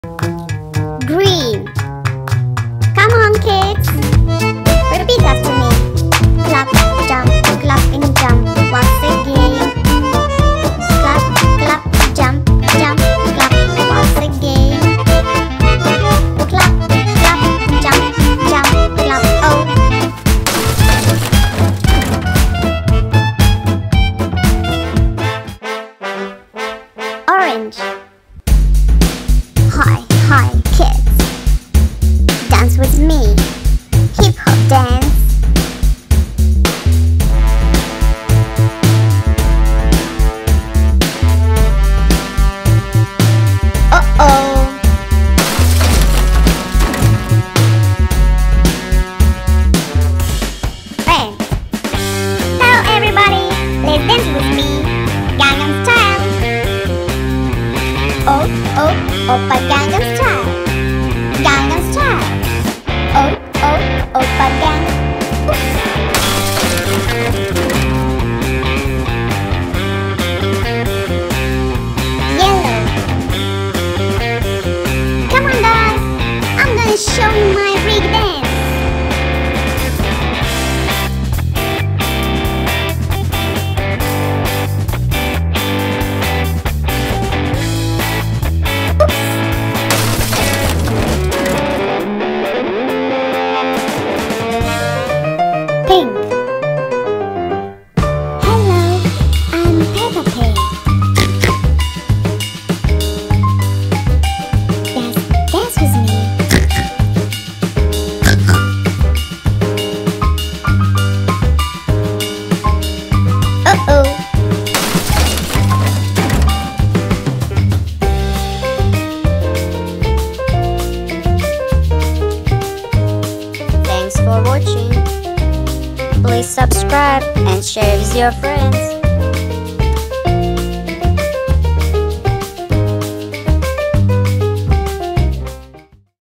Learning Colors with Balls and Character Eggs Dancing for Kids Children Toddlers
Learning Colors with Balls and Character Eggs Dancing
Toddler learning video
Surprise eggs funny dance
Fun and creatives videos for children, toddlers and babies.